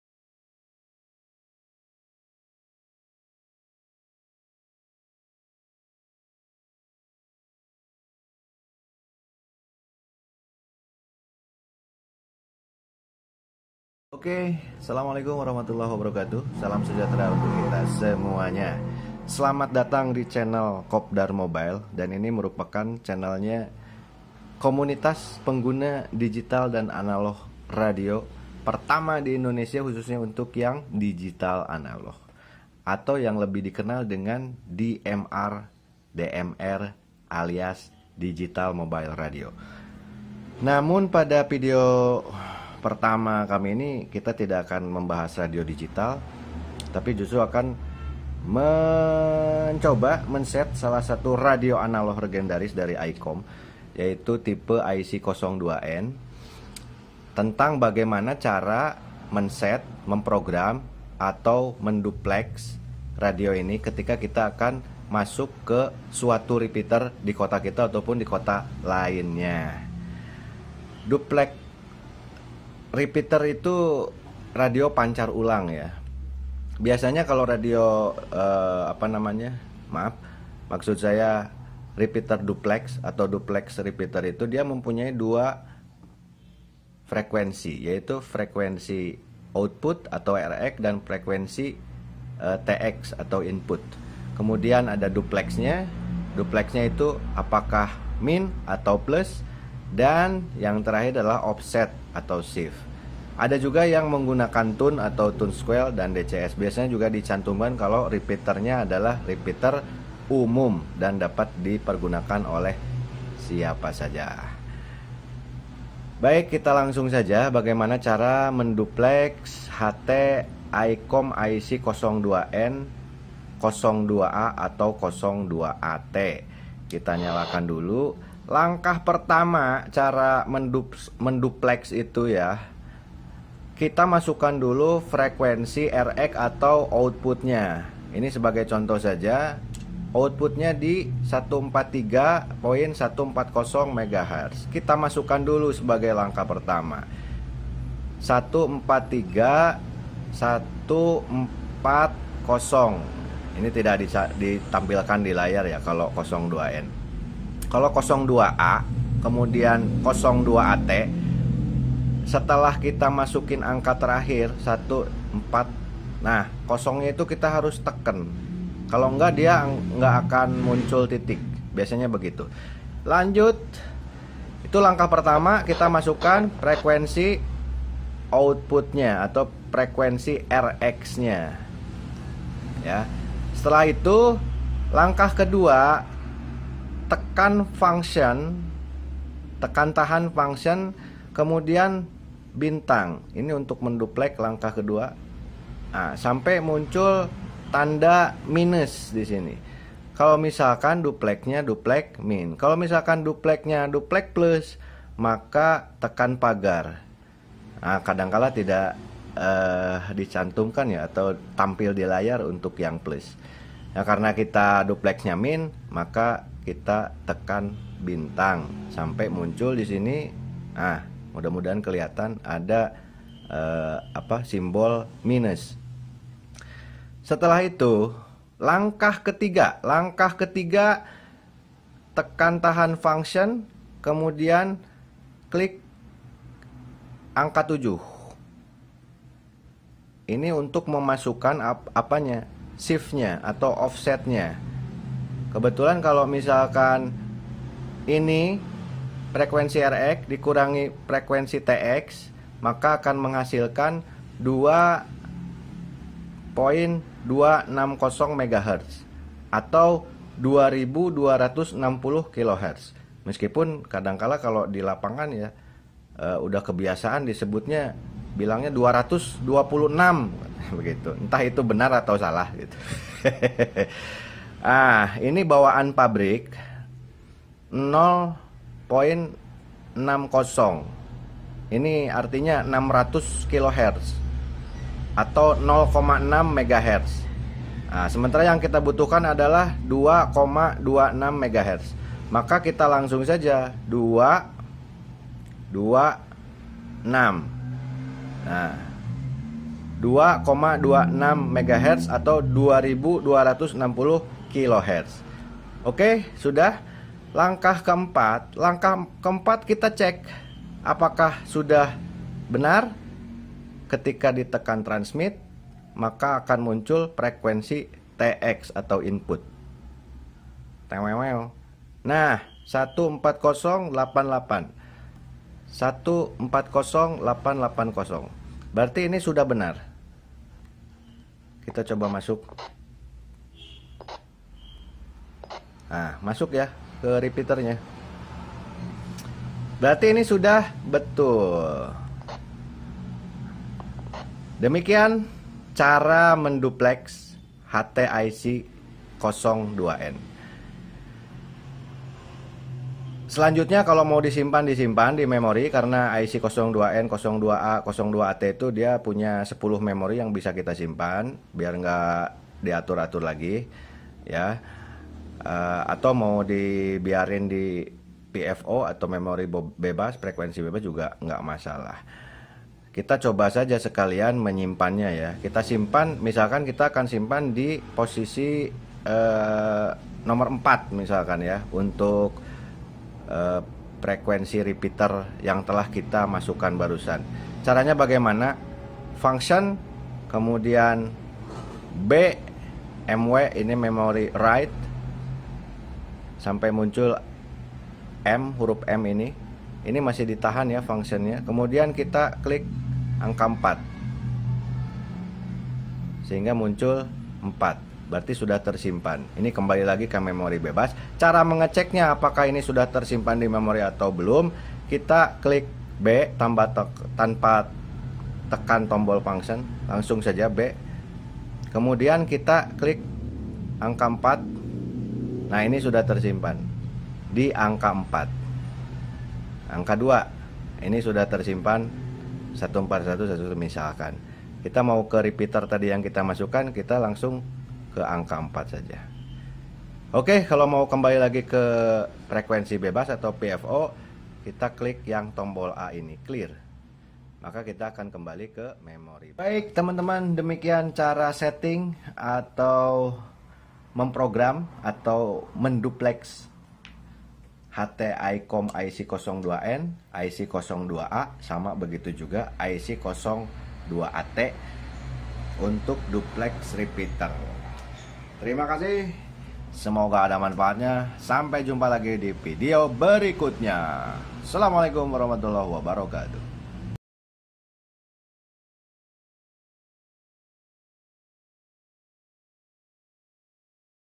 CARA SETTING DUPLEX HT ICOM IC-02N IC-02A IC-02AT
Berikut ini cara setting atau program duplex repeater unyuk HT legendaris ICOM tipe IC-02N, IC-02A, dan IC-02AT sekaligus menyimpannya kedalam memori. Semoga bermanfaat.